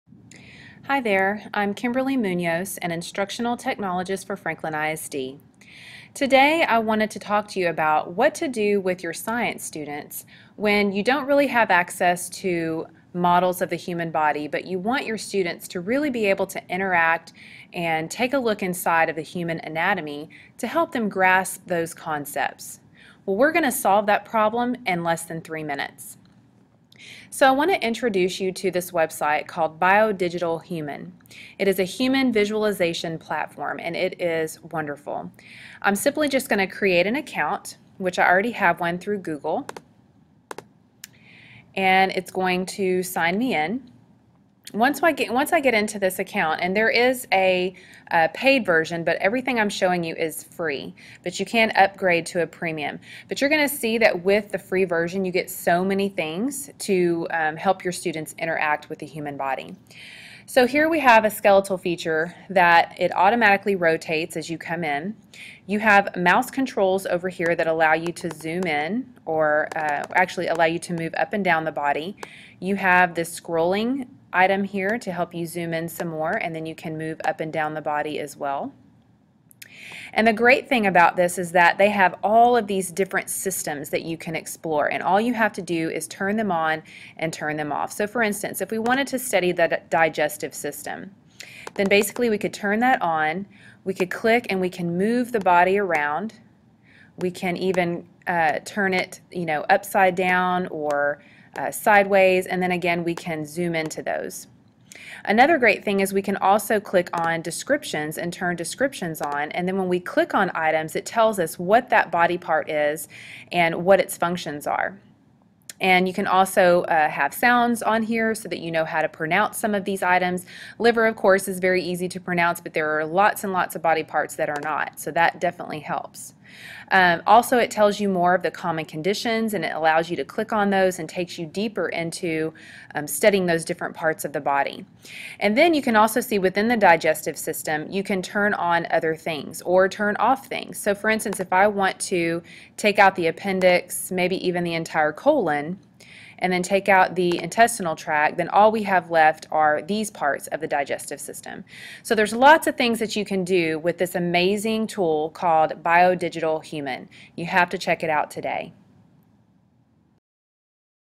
A Human Body Model for Your Classroom
With today's limited resources, it can be difficult to find top-quality resources to use with students. This is especially true when it comes to the sciences, as science resources can be particularly costly! In this 3-Minute Classroom Problem Solver, SimpleK12's Teacher Learning Community presenter Kim Munoz gives a tour of BioDigital Human, a free digital resource for you and your students to explore the human body.